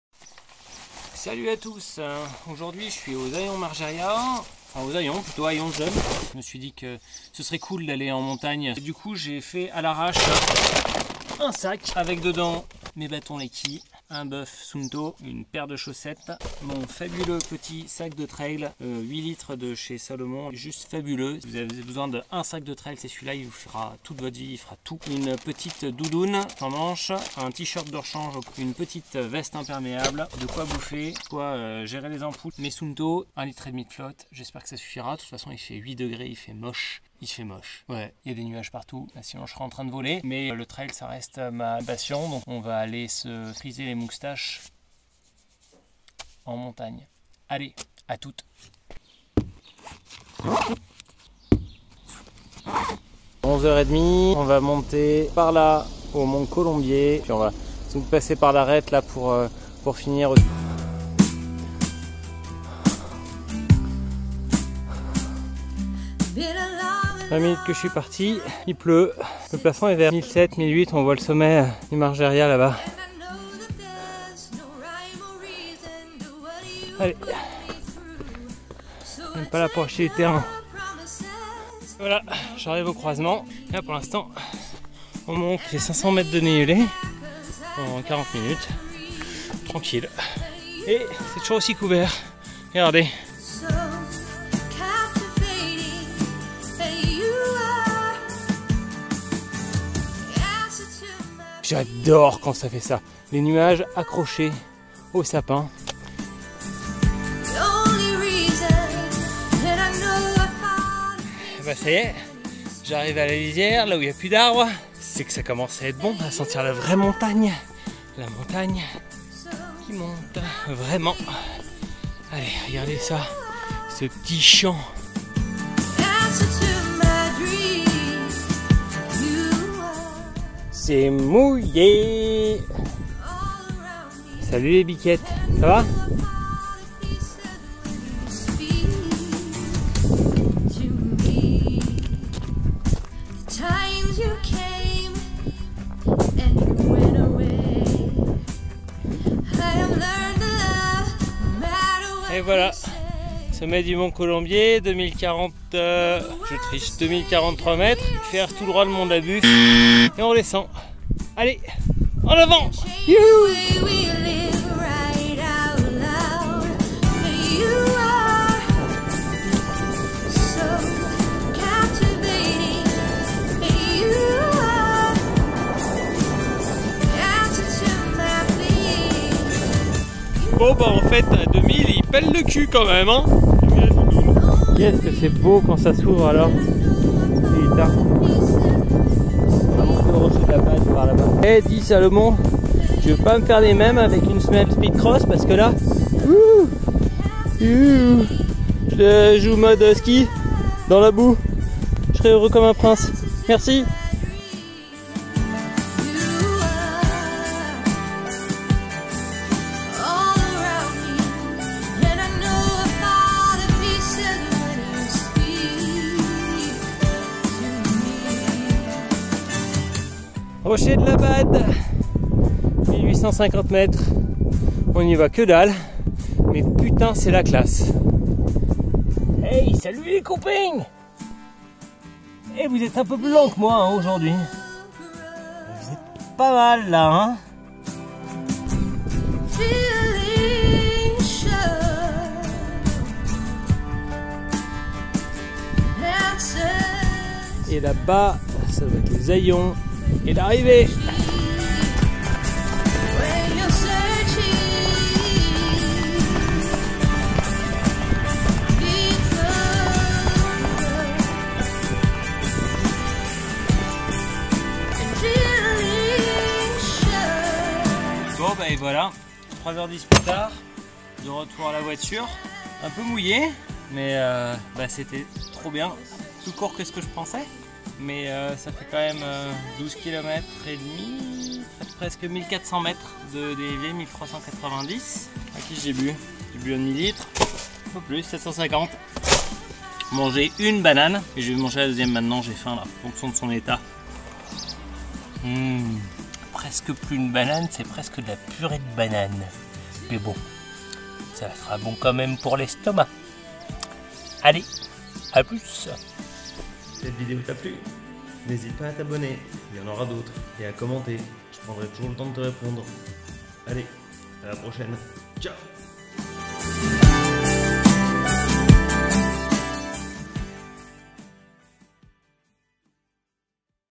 Trail Running : Mont Colombier et Rochers de la Bade
Vendredi 5 juin 2020 je suis allé me promener du côté d'Aillons le jeune pour monter au Mont Colombier. J'ai ensuite enchaîné avec l'arête des Rochers de la Bade. L'arête est sublime, mais la rando n'est pas à mettre sous tous les pieds. C'est parfois un peu technique... en tout cas c'est sublime, malgré le temps maussade !